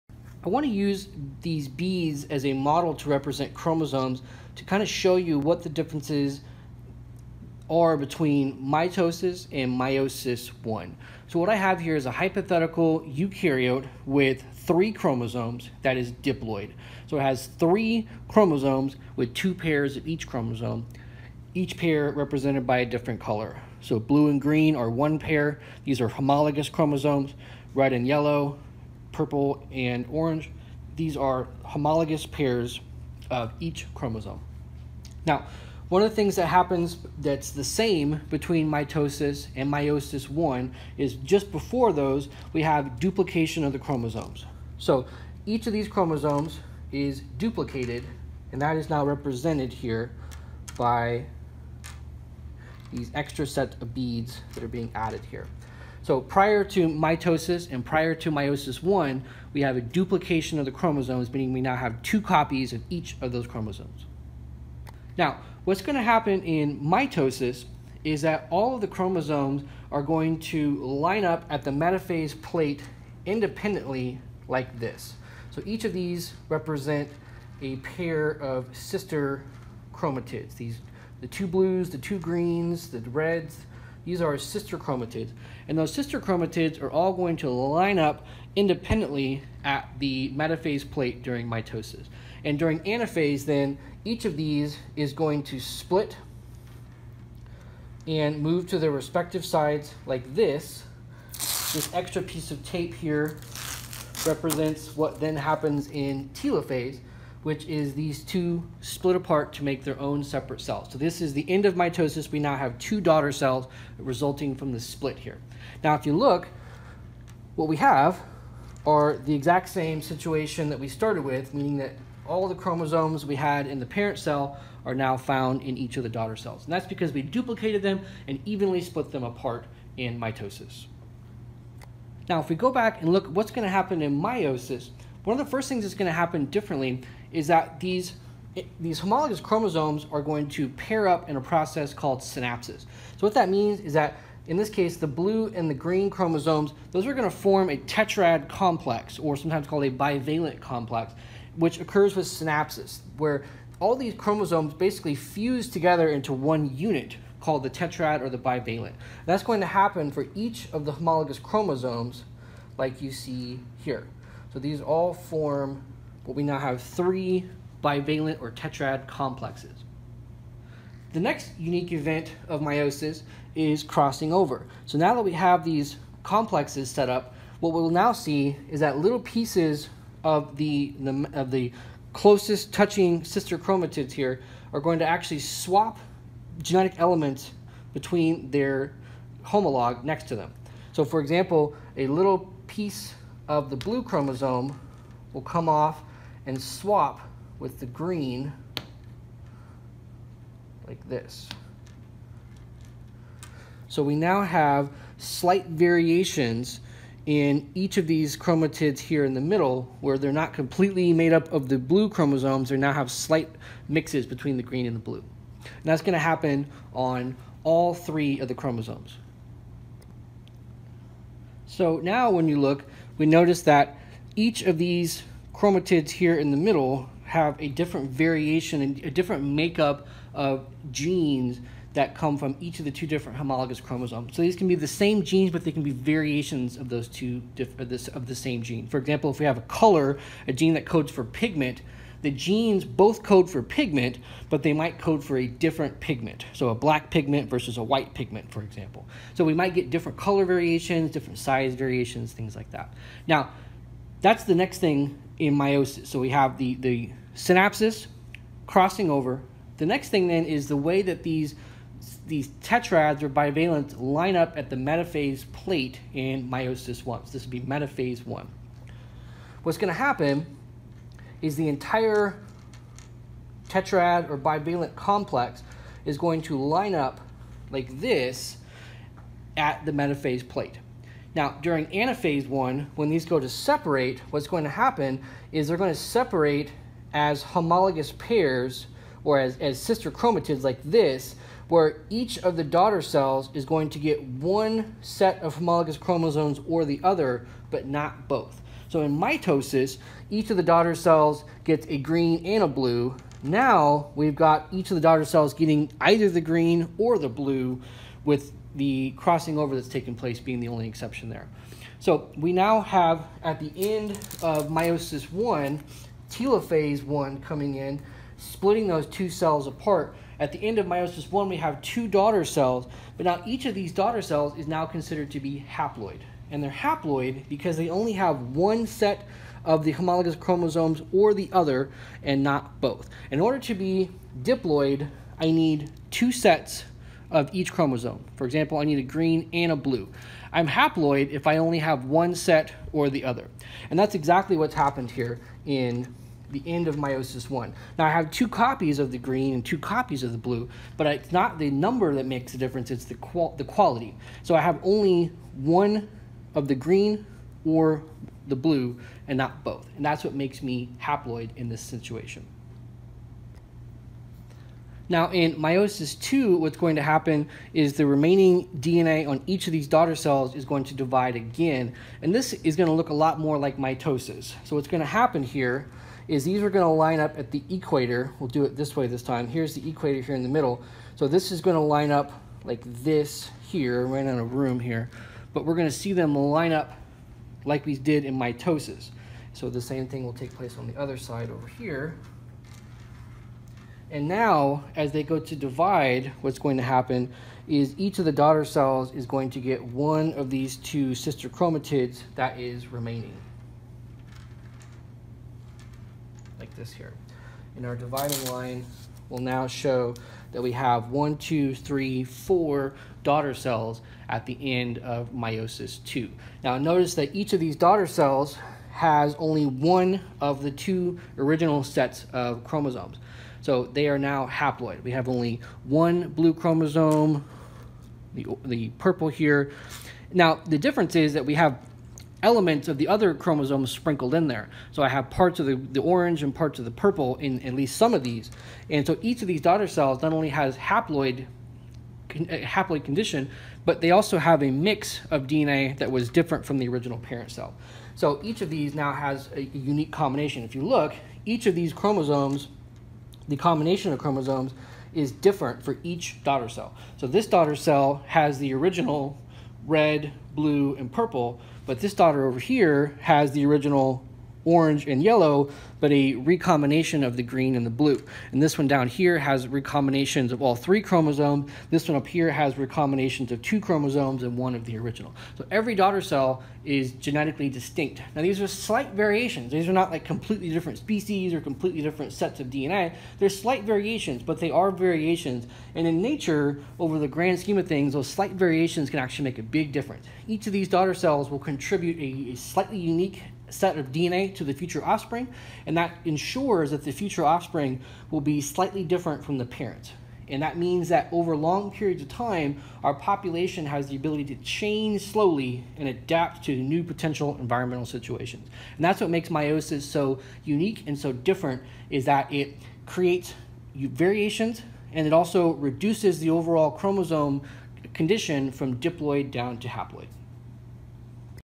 mitosis vs meiosis - simulation with beads
Here I do a very quick simulation on the key differences between mitosis and meiosis, especially focusing on the differences in meiosis I.